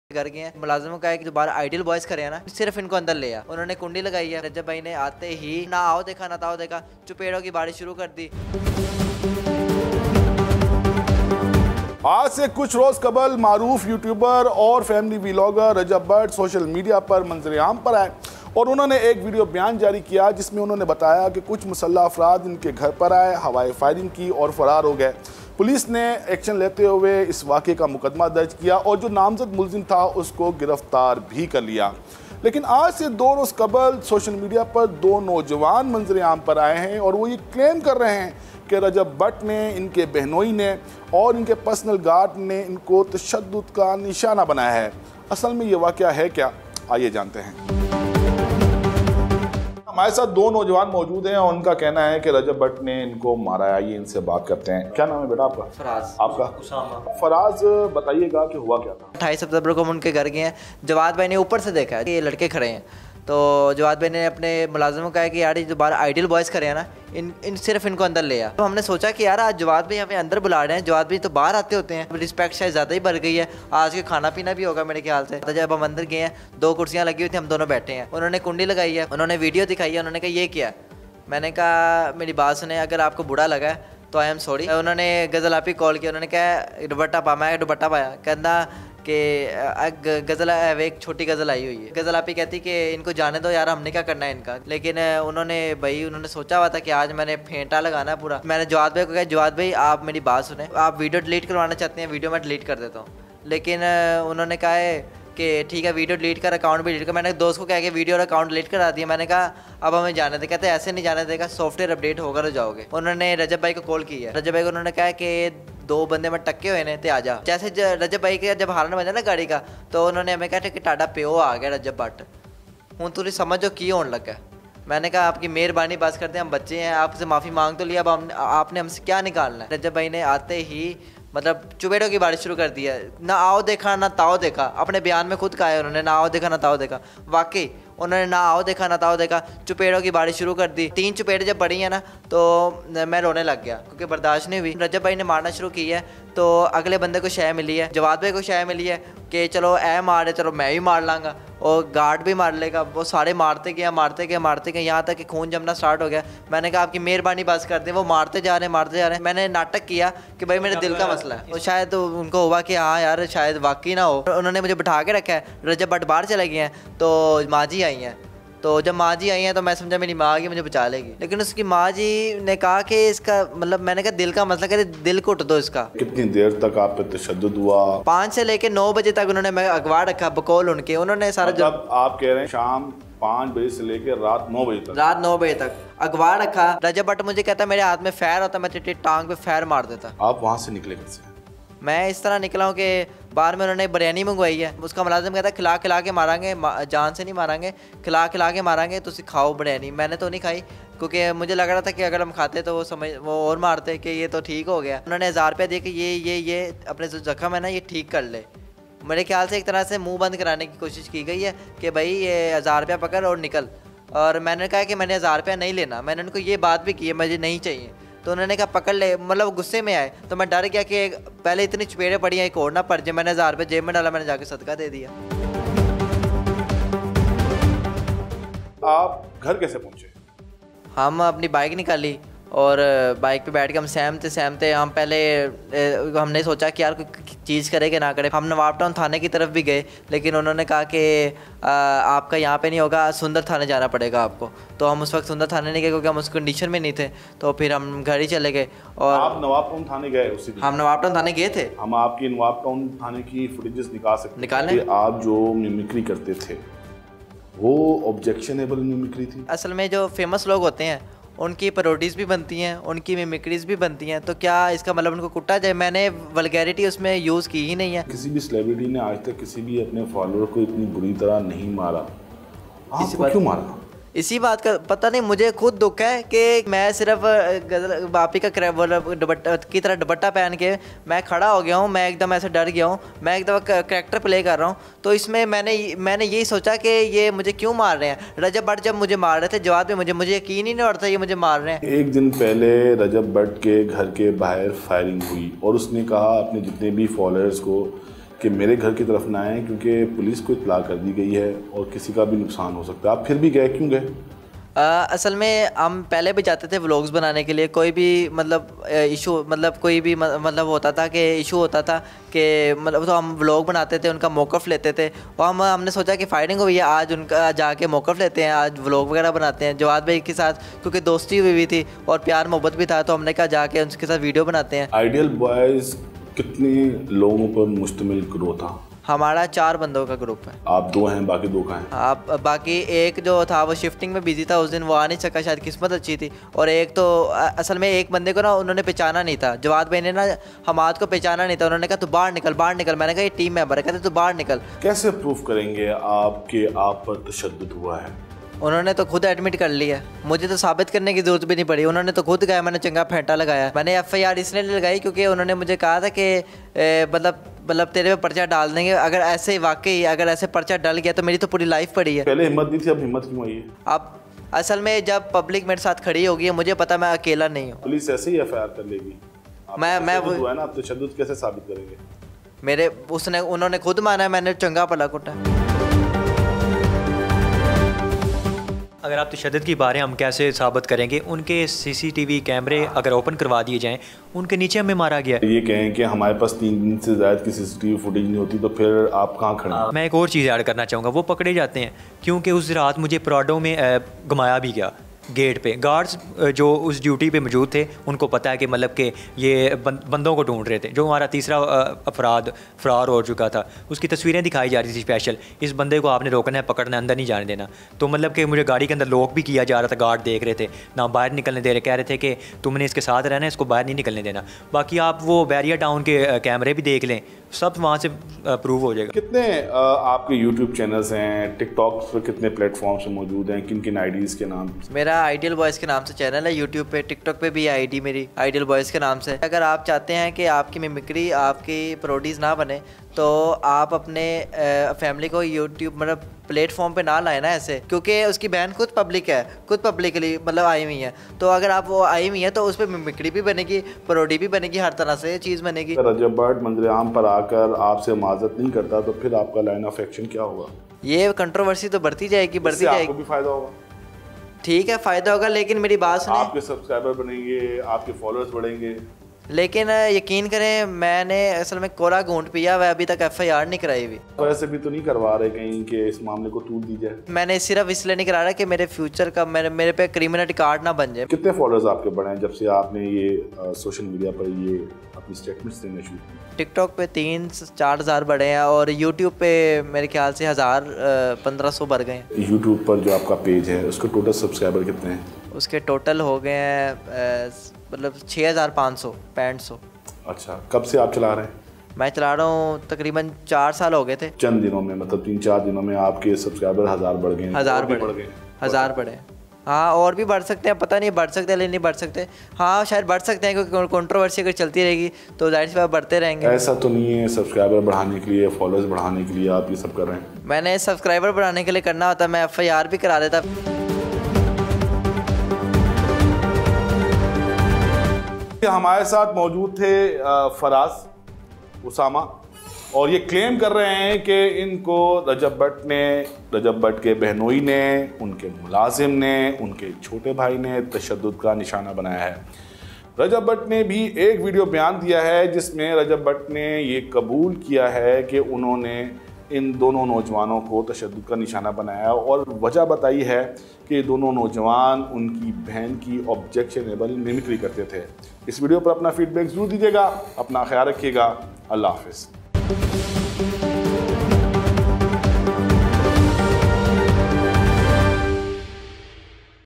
Famous Youtuber and family Vlogger Rajab Butt
Rajab Butt is a famous Youtuber and family Vlogger. A group of young Youtubers have done mimicry of Butt's family members in their Vlogs. The group was called inside his home and what happened next? Watch this video

Famous Youtuber and family Vlogger Rajab Butt exposed. Bolo Lahore Aamir Majeed | SAMAA Digital